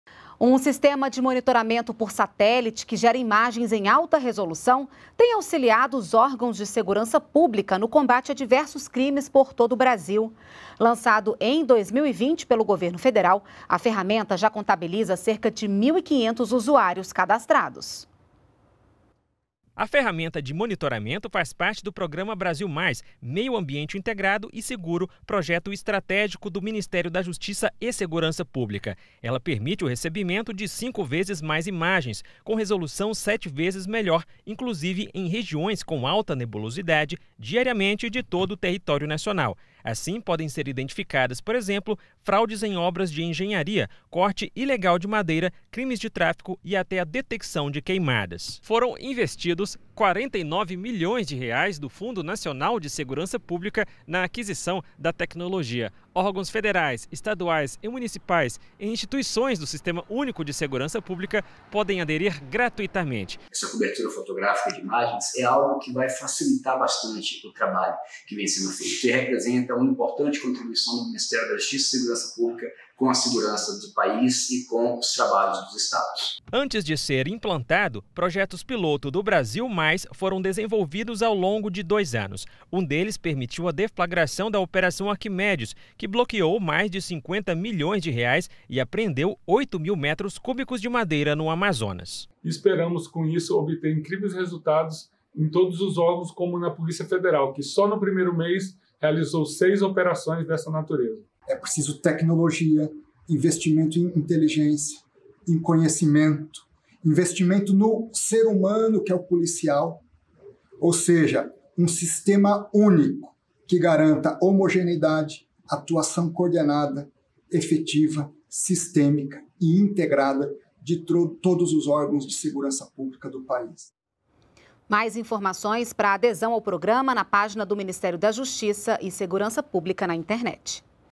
Imagens de satélite de alta resolução reforçam o combate à criminalidade em todo o país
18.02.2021 - Um sistema de monitoramento por satélite, que gera imagens em alta resolução, tem auxiliado os órgãos de segurança pública no combate a diversos crimes por todo o Brasil. Lançado em 2020 pelo Governo Federal, a ferramenta já contabiliza cerca de 1,5 mil usuários cadastrados.